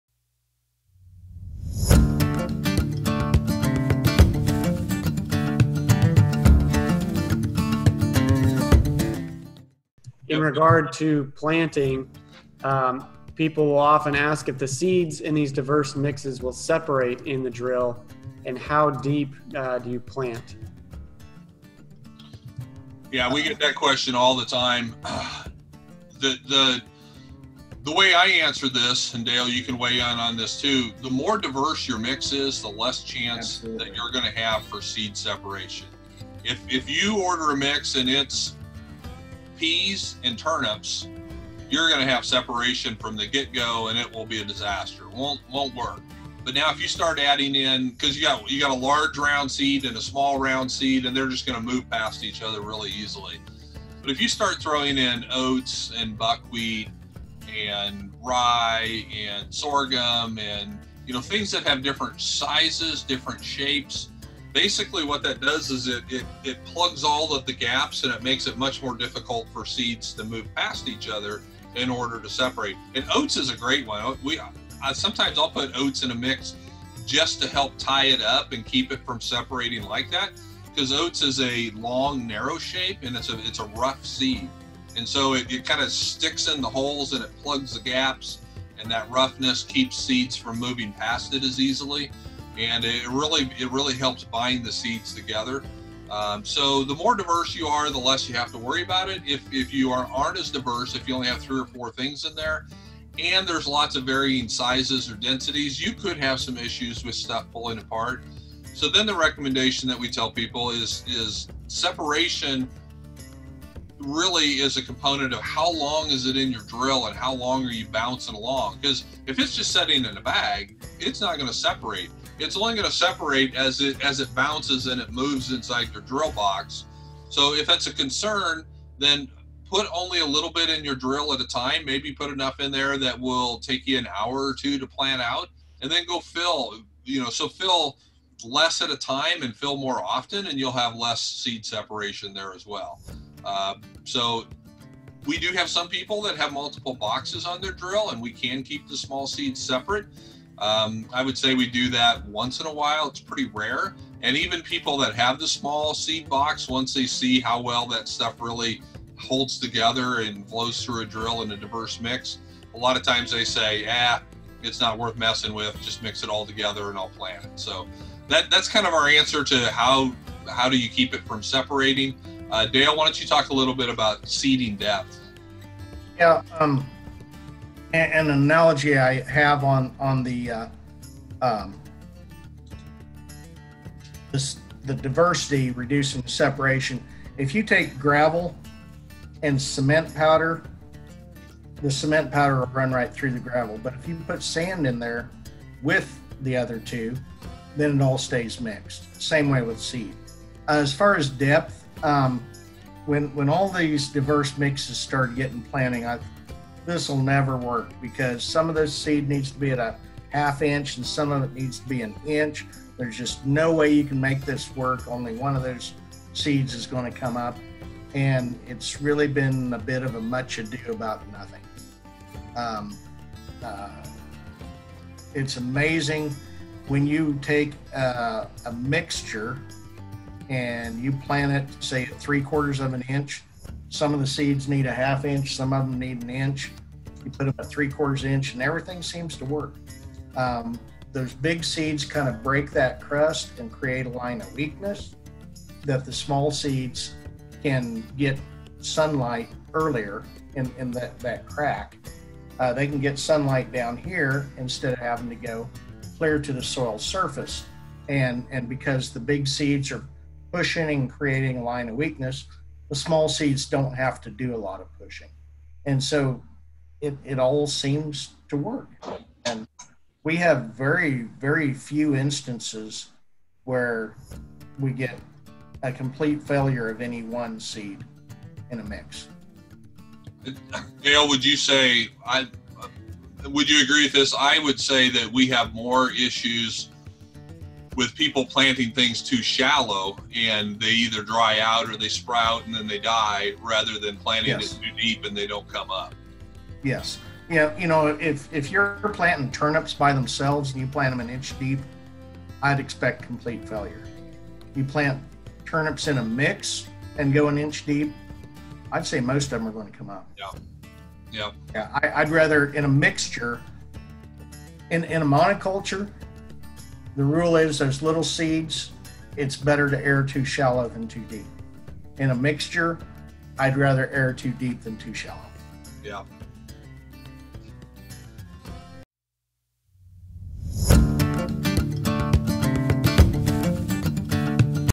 FAQ - Will the seed separate in the planting boxes?
Keith and Dale discuss how to keep your seeds from separating.